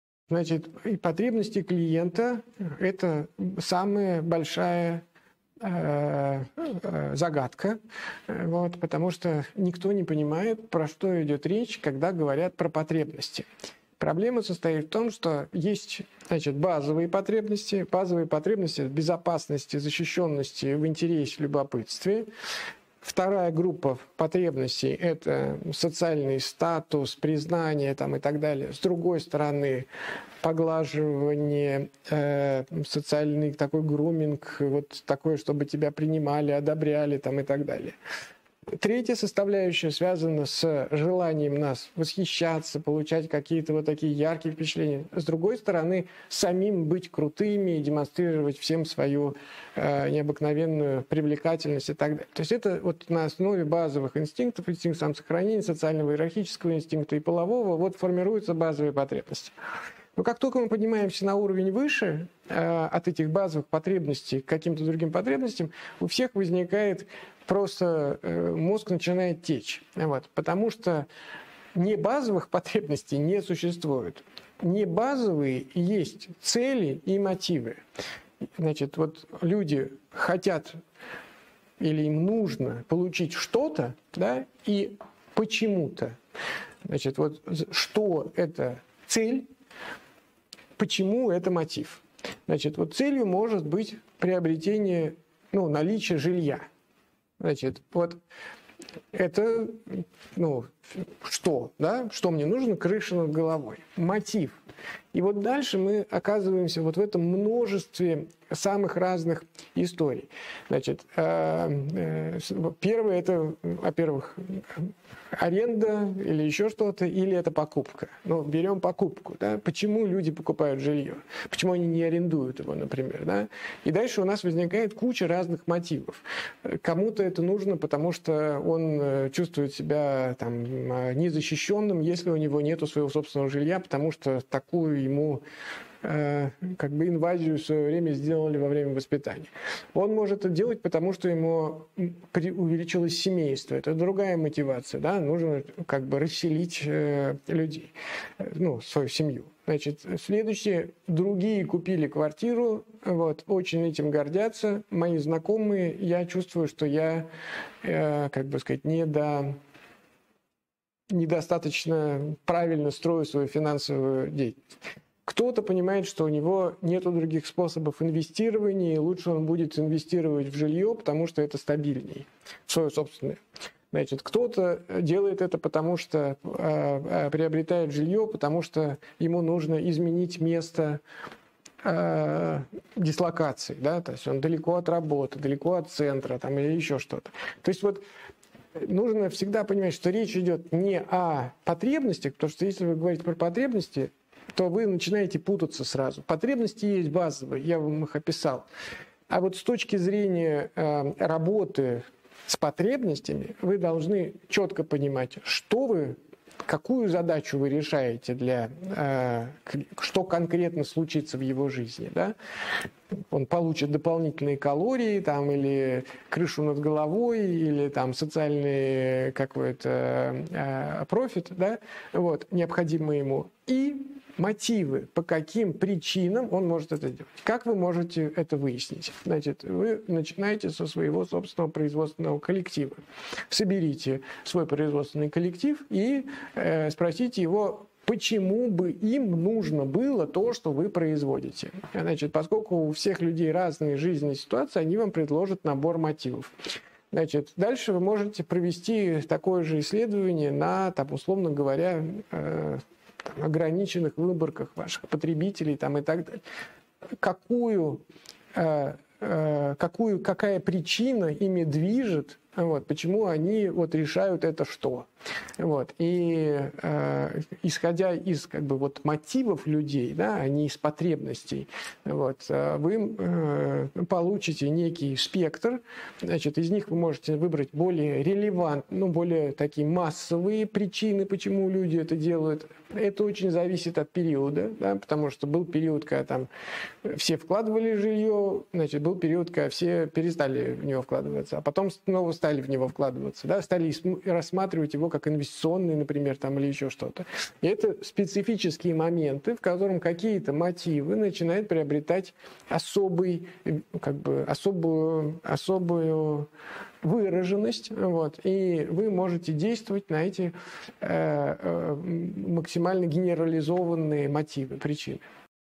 Как понять цели и мотивы клиентов? 👥 | Андрей Курпатов | Мозг и Бизнес
brain_and_business: Как понять цели и мотивы клиентов? 👥

Потребности клиентов - самая большая загадка.

Потому, что никто не понимает про что идет речь, когда говорят про потребности. 🧐

Давайте разберемся вместе!

Существуют базовые инстинкты, из которых формируются базовые потребности (безопасность, любовь, уважение, любопытство т.д.)

Проблема состоит в том, что цели и мотивы НЕ относятся к базовым потребностям, но должны их закрывать.

Поэтому, для того, чтобы понять цели и мотивы своего клиента вы должны руководствоваться не самими базовыми потребностями, а решениями, которые их закрывают. 🎯

А для этого, вам нужно:

1. Понять, что хочет получить ваш клиент (какая у него цель?);

2. Понять, что движет им, в принятии решения приобрести ваш продукт (какой у него мотив?).

💬 Подробнее о том, как определять цели и мотивы клиентов, Андрей Курпатов рассказал в видеоролике.